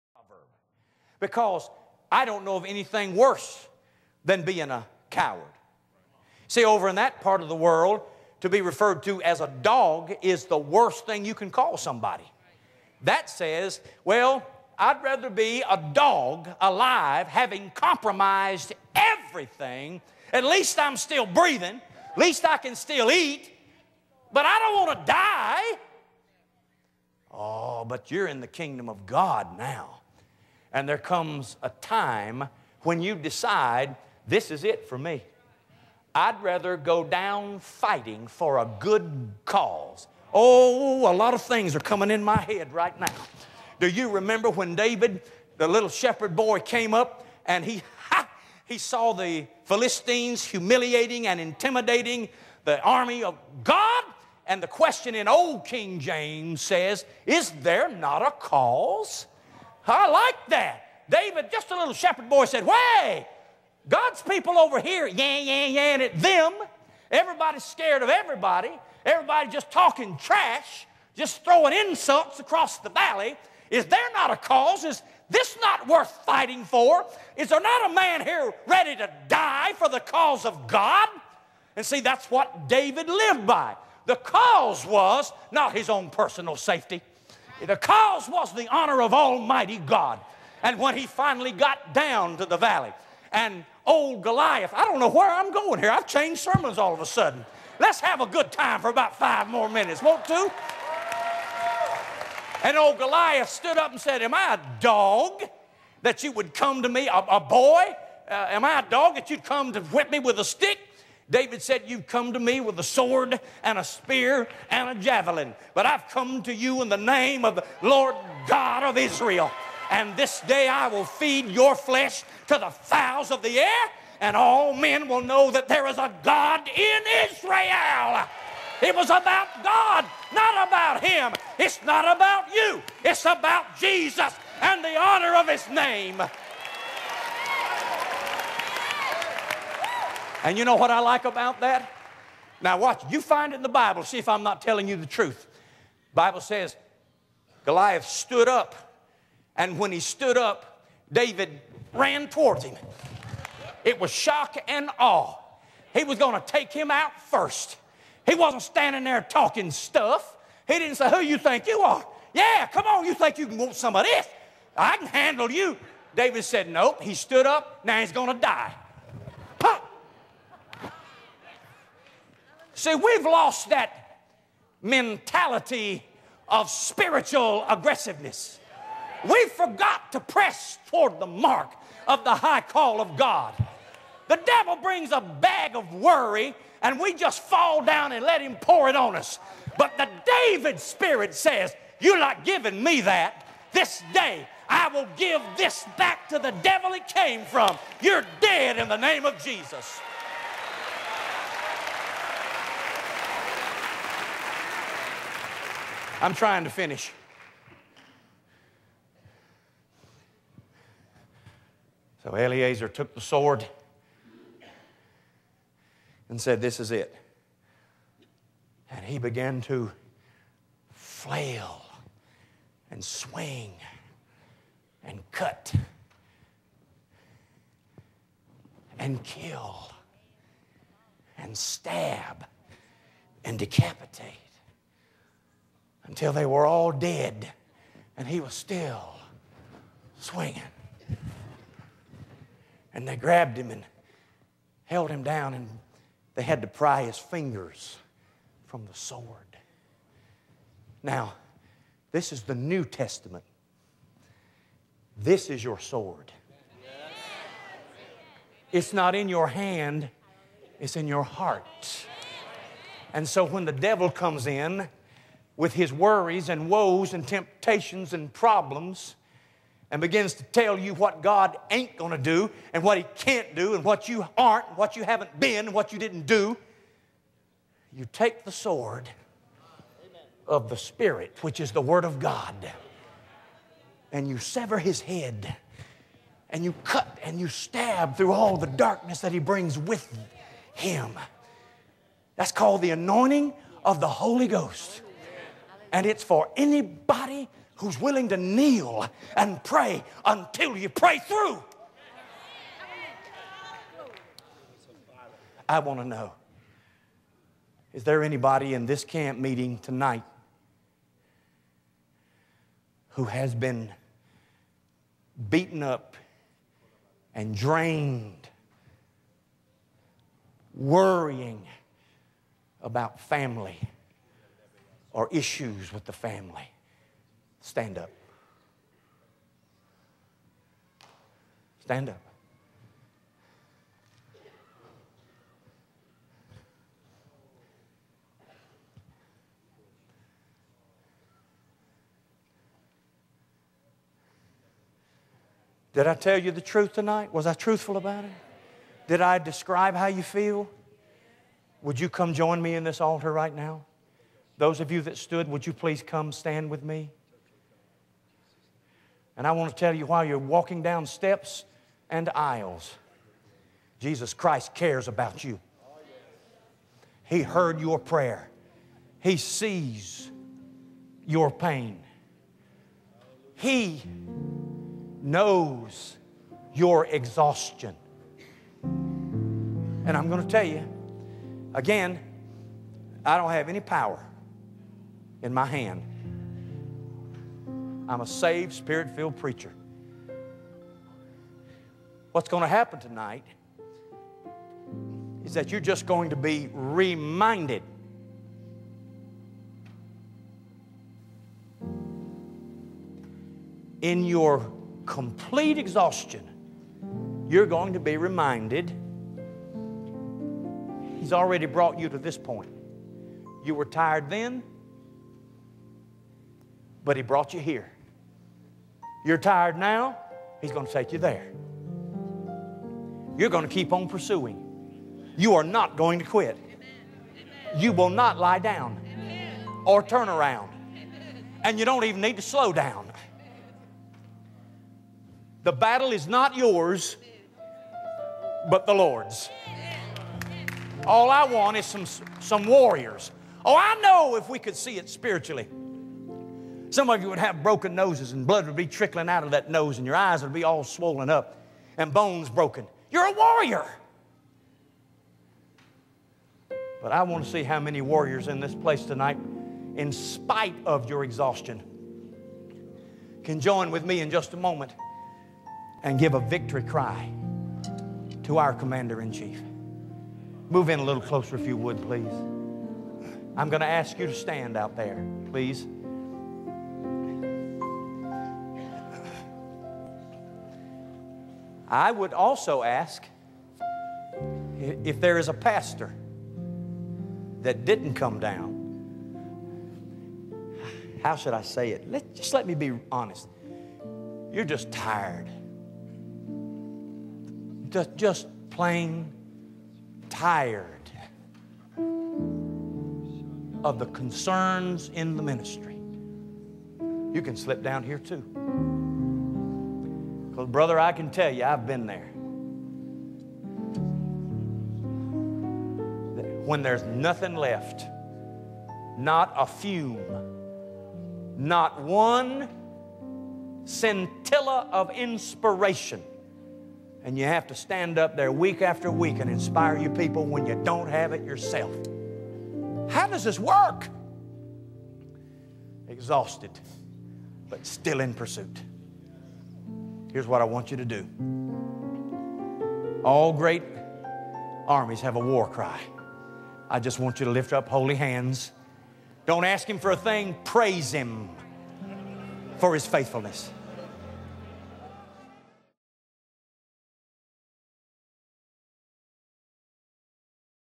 LORAN LIVINGSTON --- (Part 7) -- 'Exhausted, But Still Pursuing'
LORAN LIVINGSTON, pastor of the Central Church of God in Charlotte, North Carolina, ministers to the vast crowd at the 2009 Tennessee Church of God Camp Meeting. His sermon that evening was entitled: "Exhausted, But Still Pursuing". You'll want to listen to each part of this anointed message.